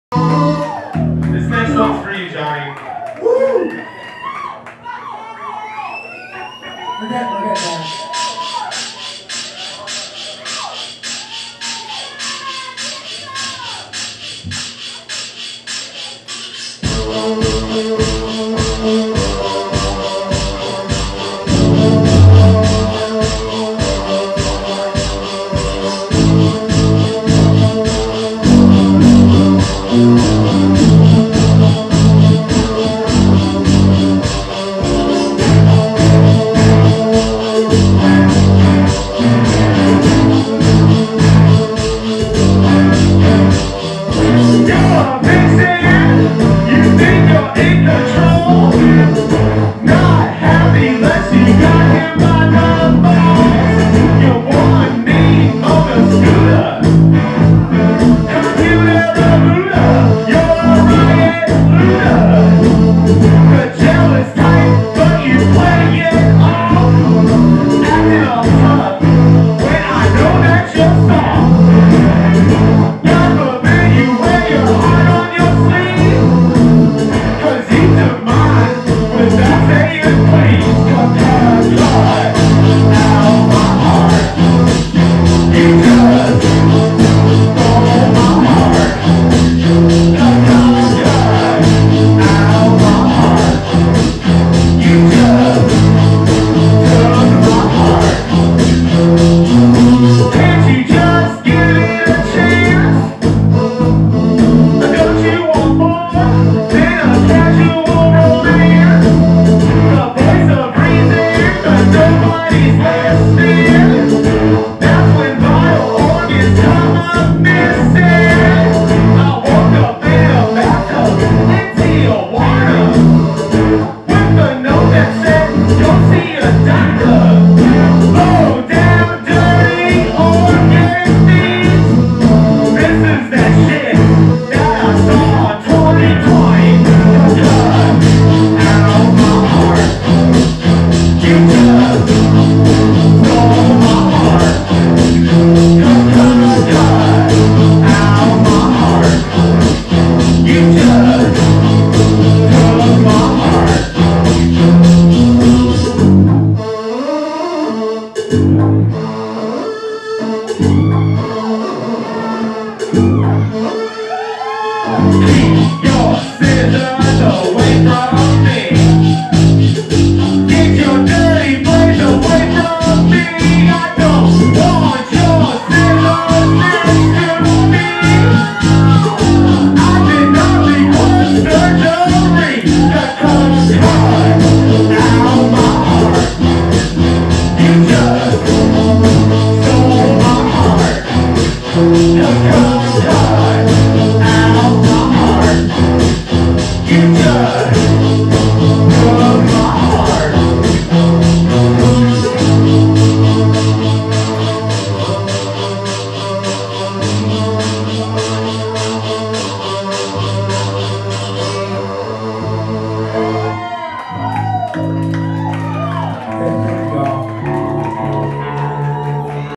Neapolitan Beard 4
Neapolitan Beard Live @ John's Benefit Show with Lucky Lucy. Ash St. Saloon 7/19/2008